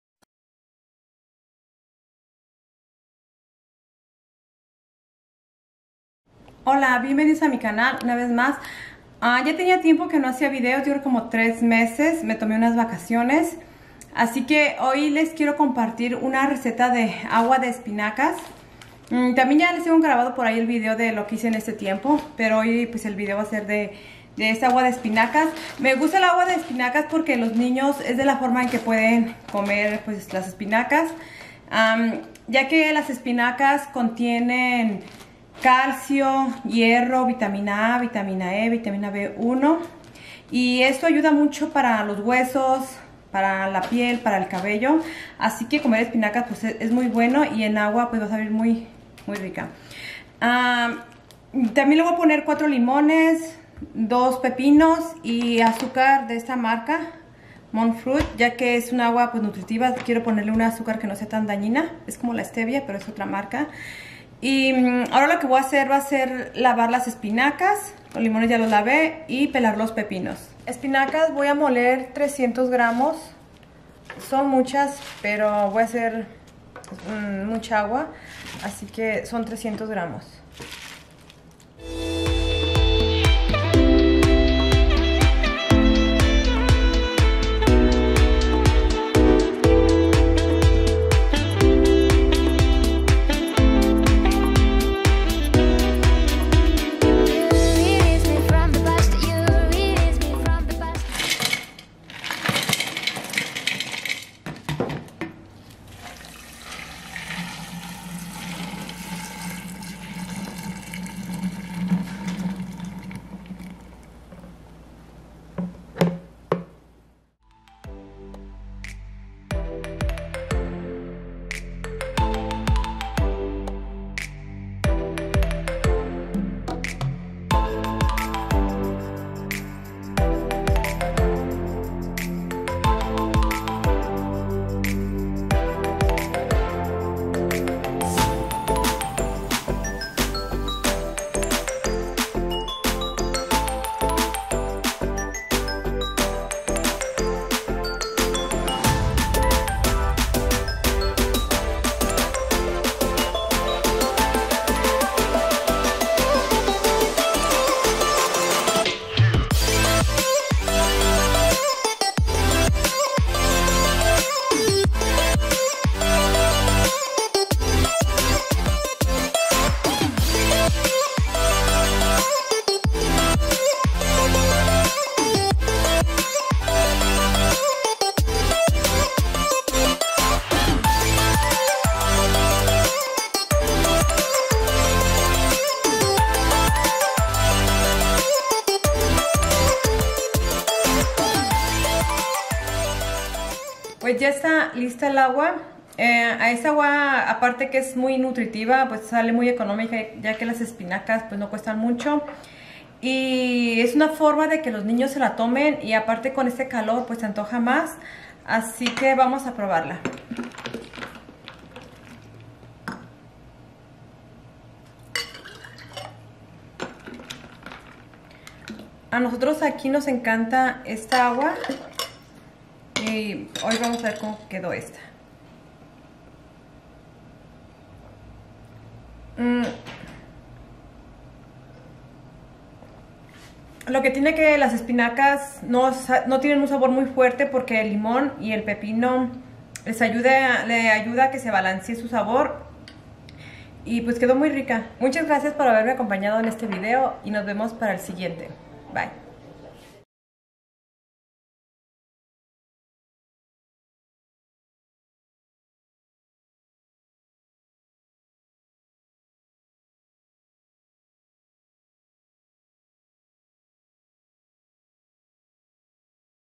Agua de Espinacas que ayuda para la Piel cabello y Huesos 💪
ingredientes:
300 gramos de espinacas
2 pepinos
4 limones
80 gramos azucar (monk fruit sweetner)
4 litres de agua